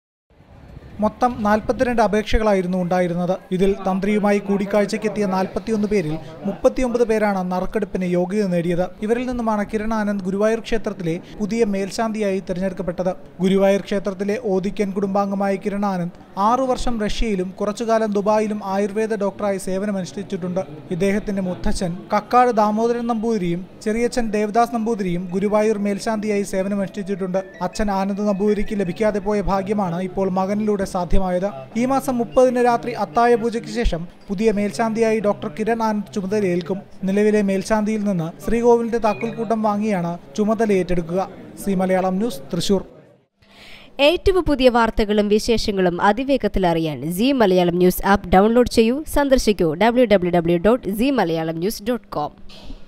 ഗുരുവായൂർ മേൽശാന്തിയായി കിരൺ ആനന്ദ് തെരഞ്ഞെടുക്കപ്പെട്ടു | Guruvayoor News | Latest News
ഗുരുവായൂർ മേൽശാന്തിയായി കിരൺ ആനന്ദ് തെരഞ്ഞെടുക്കപ്പെട്ടു